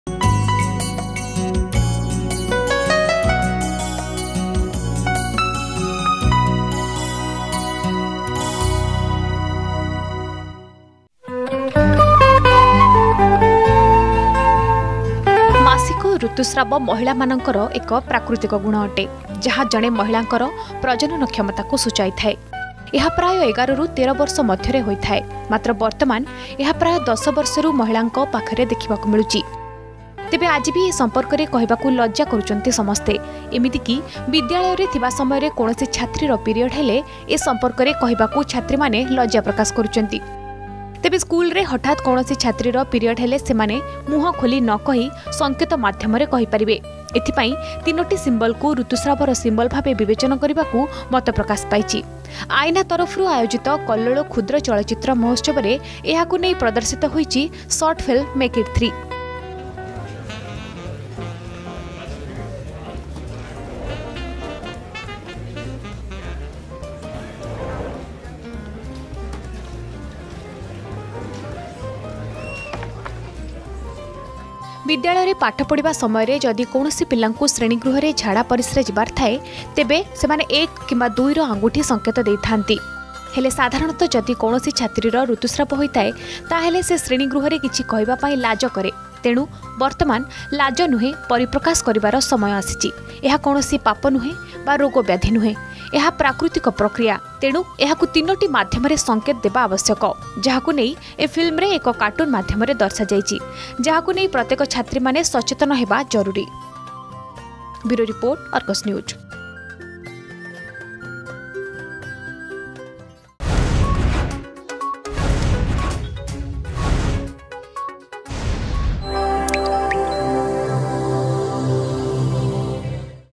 ସ୍କୁଲରେ ଋତୁସ୍ରାବ ସମସ୍ୟା
ବିଦ୍ୟାଳୟରେ ପାଠ ପଢ଼ିବା ସମୟରେ ଯଦି କୌଣସି ଛାତ୍ରୀର ଋତୁସ୍ରାବ ହୋଇଥାଏ ତାହେଲେ ସେ ଶ୍ରେଣୀ ଗୃହରେ କିଛି କହିବା ପାଇଁ ଲାଜ କରେ ତେଣୁ ବର୍ତ୍ତମାନ ଲାଜ ନୁହେଁ ପରିପ୍ରକାଶ କରିବାର ସମୟ ଆସିଛି। ତେଣୁ ଏହାକୁ ୩ ମାଧ୍ୟମରେ ସଂକେତ ଦେବା ଆବଶ୍ୟକ ବୋଲି ମତ ପ୍ରକାଶ ପାଇଛି।